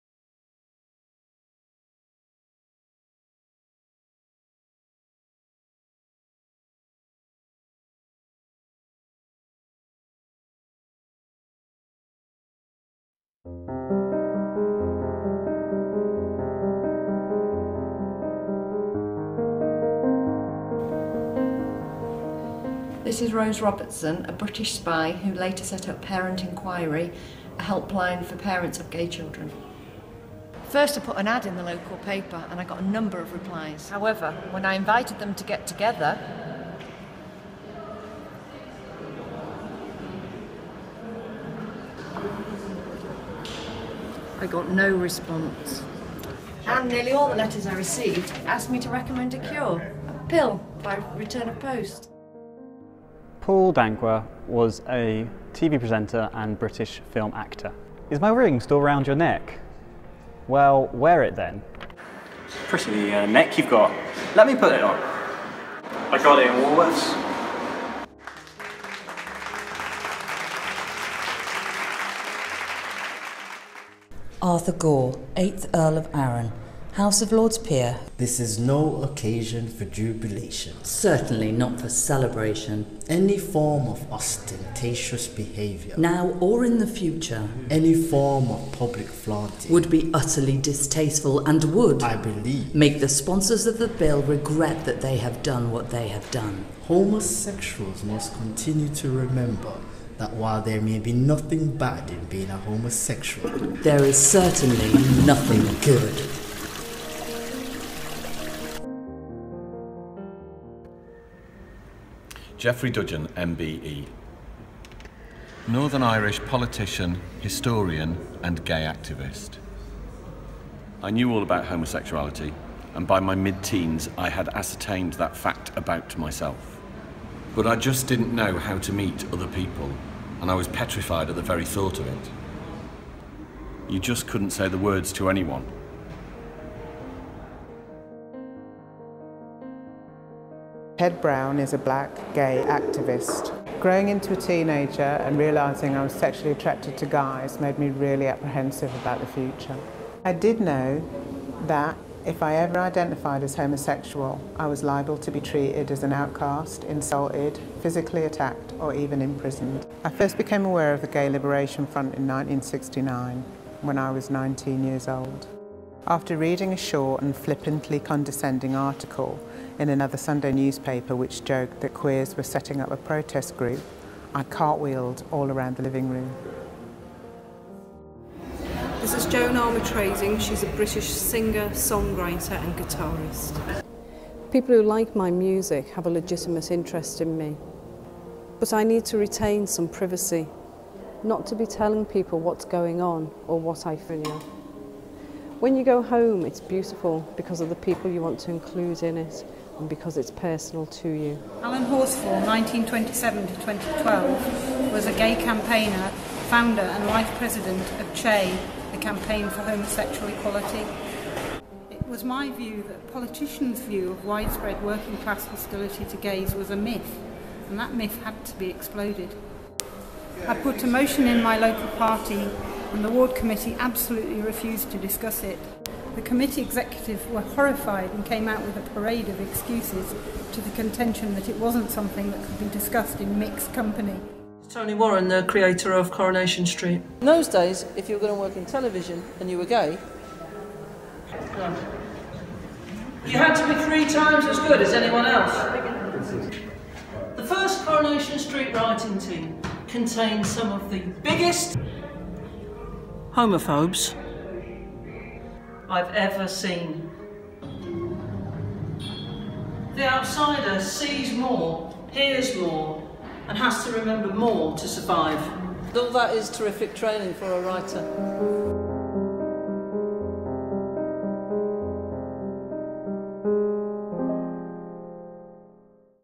Ostentatious Behaviour
Celebrating the lives of LGBT+ people and their supportive allies who were alive in 1967 using their own words to commemorate fifty years since the Sexual Offences Act partially decriminalised homosexuality in England and Wales. All filmed on mobile phones by beginner filmmakers at the People’s History Museum, Manchester as part of MobDoc LGBT Mobile Filmmaking Workshop hosted by Queer Media and Life’s a Drag. QueerMedia.org.uk